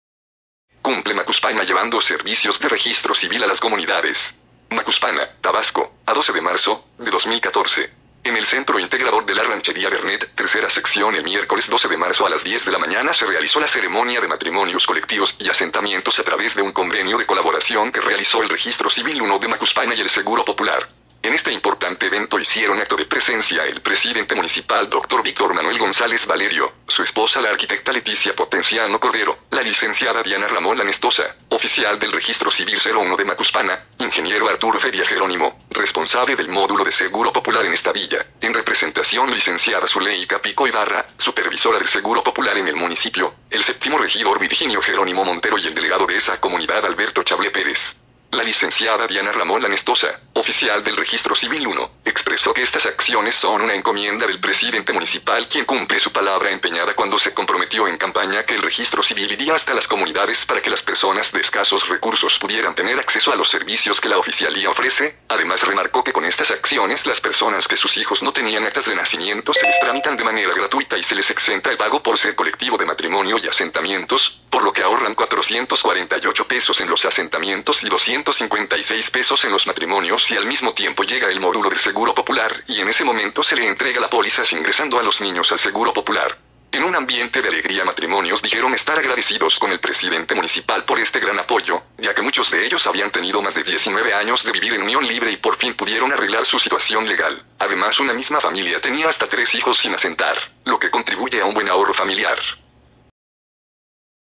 Cumple Macuspana llevando servicios de Registro Civil a las comunidades
Macuspana, Tabasco, a 12 de marzo de 2014.
En el centro integrador de la ranchería Vernet tercera Sección el miércoles 12 de marzo a las diez de la mañana se realizó la ceremonia de matrimonios colectivos y asentamientos a través de un convenio de colaboración que realizó el registro civil 1 de Macuspana y el seguro popular.
En este importante evento hicieron acto de presencia el presidente municipal Dr. Víctor Manuel González Valerio, su esposa la arquitecta Leticia Potenciano Cordero, la Licenciada Diana Ramón Lanestosa, oficial del registro Civil 01 de Macuspana, Ing. Arturo Feria Gerónimo, responsable del módulo de seguro popular en esta villa, en representación Licenciada Suleyka Pico Ibarra, supervisora del seguro popular en el municipio, el séptimo regidor Virginio Gerónimo Montero y el delegado de esa comunidad Alberto Chablé Pérez.
La Licenciada Diana Ramón Lanestosa, oficial del registro Civil 1, expresó que estas acciones son una encomienda del presidente municipal quien cumple su palabra empeñada cuando se comprometió en campaña que el registro civil iría hasta las comunidades para que las personas de escasos recursos pudieran tener acceso a los servicios que la oficialía ofrece, además remarcó que con estas acciones las personas que sus hijos no tenían actas de nacimiento se les tramitan de manera gratuita y se les exenta el pago por ser colectivo de matrimonio y asentamientos, por lo que ahorran 448 pesos en los asentamientos y 256 pesos en los matrimonios y al mismo tiempo llega el modulo del seguro popular  y en ese momento se le entrega la pólizas ingresando a los niños al seguro popular.
En un ambiente de alegría Matrimonios dijeron estar agradecidos con el presidente municipal por este gran apoyo, ya que muchos de ellos habían tenido más de 19 años de vivir en unión libre y por fin pudieron arreglar su situación legal, además una misma familia tenía hasta tres hijos sin asentar, lo que contribuye a un buen ahorro familiar.